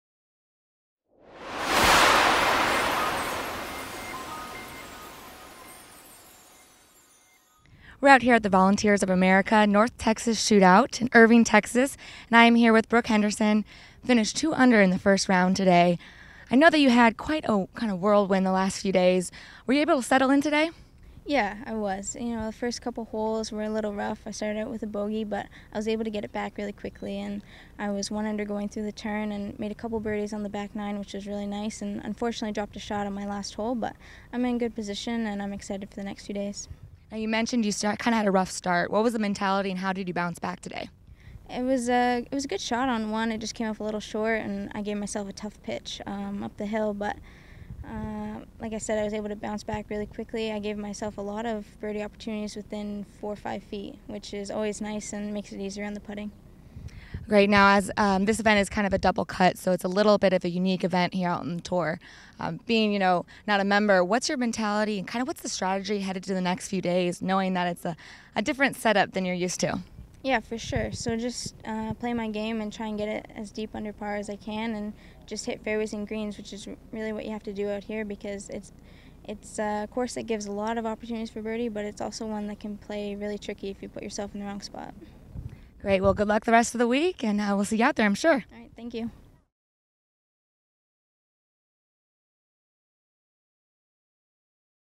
Brooke Henderson First Round Interview at North Texas
Henderson talks about getting settled into her game at North Texas.